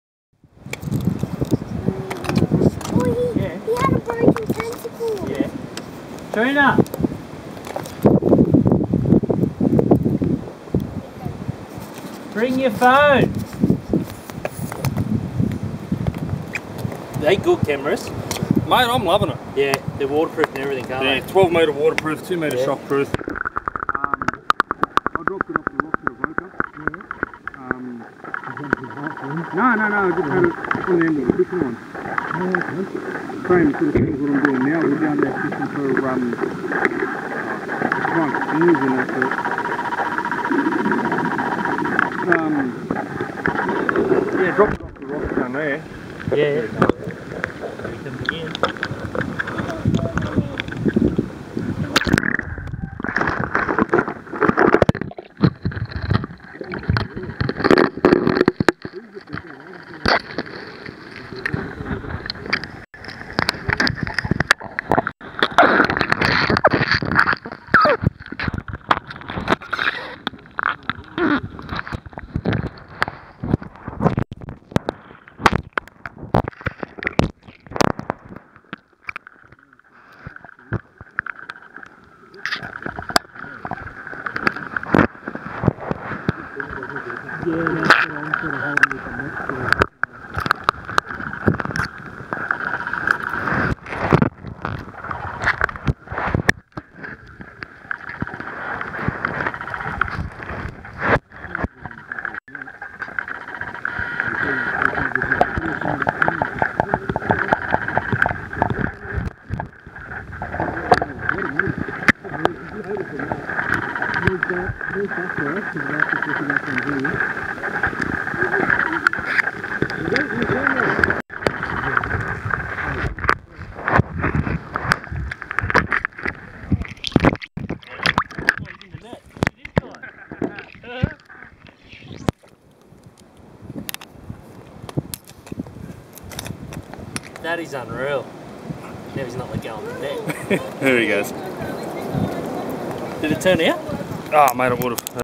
Octopus at Norah Head
Octopus trying to steal my Panasonic Lumix FT3 at Norah Head